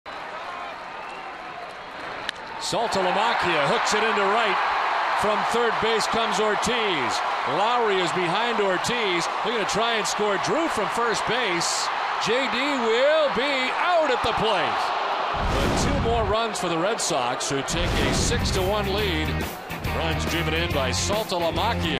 Saltalamacchia's two-run single
TOR@BOS: Saltalamacchia drills a two-run single